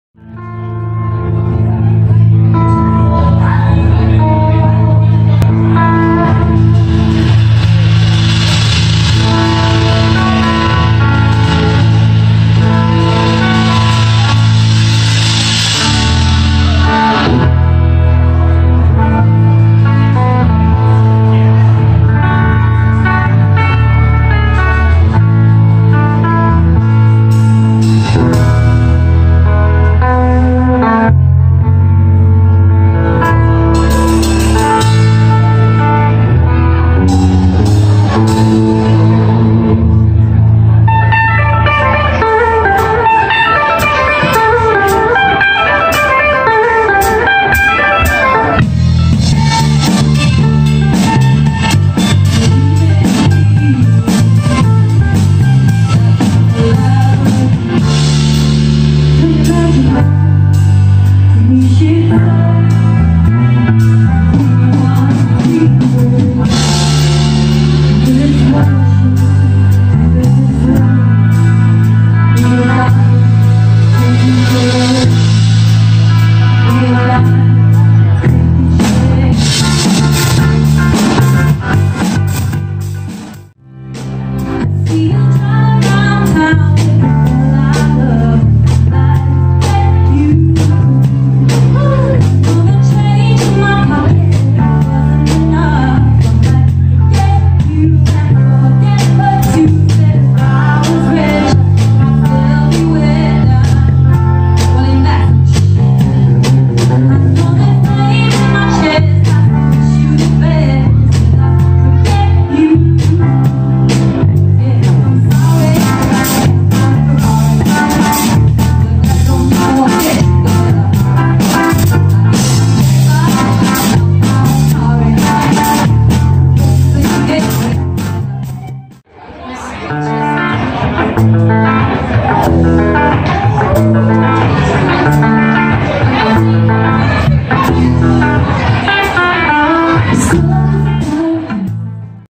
The Dancing Monkeys stage cam Xmas party 2018
A fan kindly filmed us side of the stage at an Xmas party gig 22nd December 2018! Just for fun xx